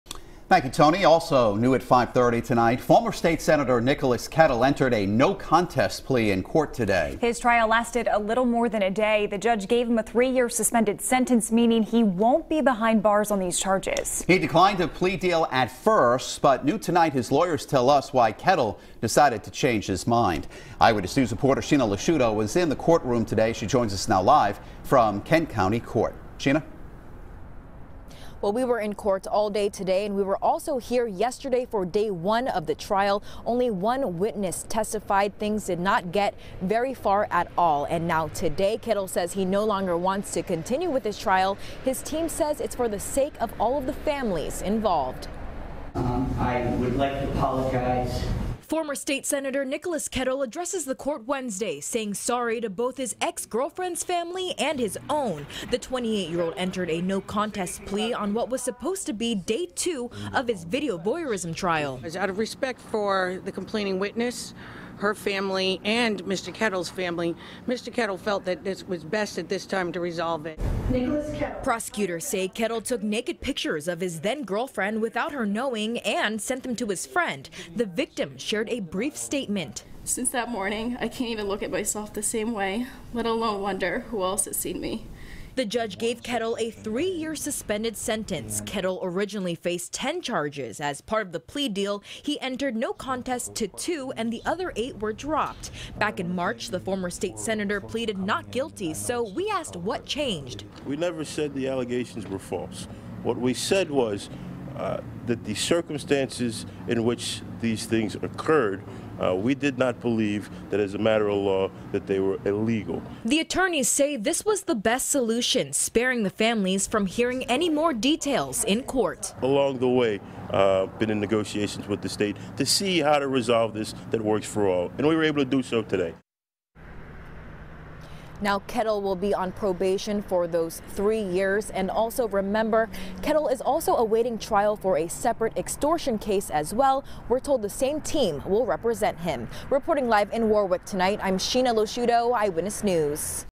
Kettle avoids prison time in video voyeurism case
The video voyeurism trial of former Rhode Island state Sen. Nicholas Kettle came to an abrupt end Wednesday after he and his defense team decided to accept a plea deal.